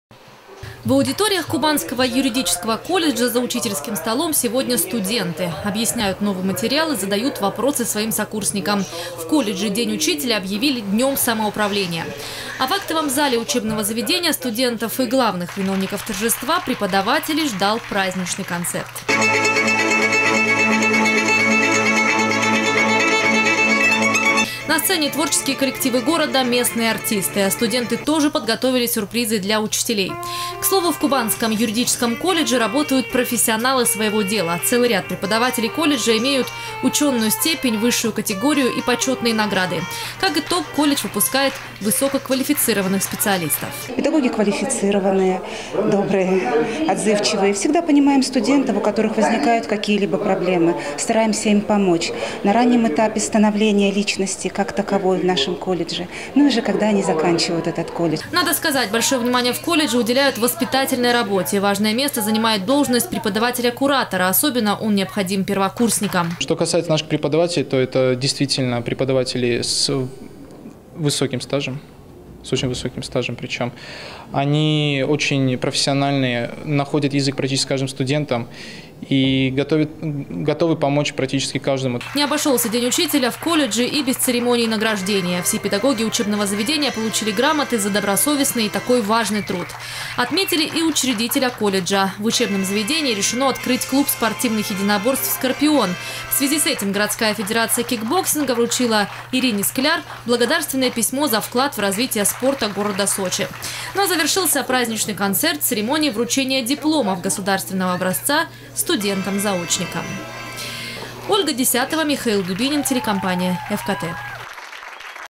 Педагоги юридического колледжа в Сочи отметили профессиональный праздник. Новости Эфкате
С праздником педагогов сегодня поздравляли также в высших и средне-специальных учебных заведениях. Так, в Кубанском Юридическом колледже к профессиональному празднику студенты для учителей подготовили праздничный концерт.
Сочи. Новости.
Телеканал Эфкате.